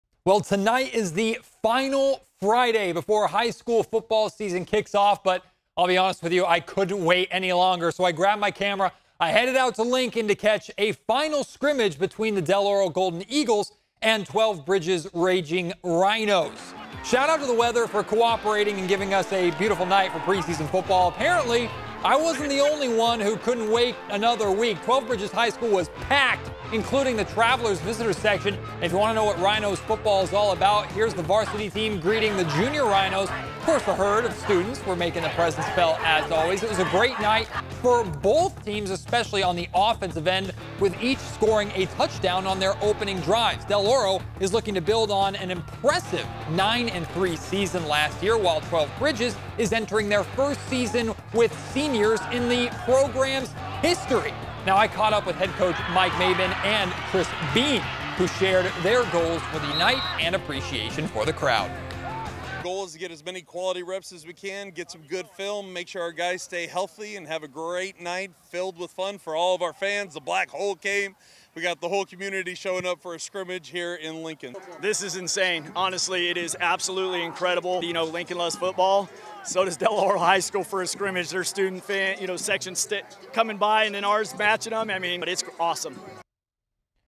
Del Oro, Twelve Bridges meet for preseason scrimmage
The stadium was packed from the home to visitor section, showing people are ready for the high school football season to start.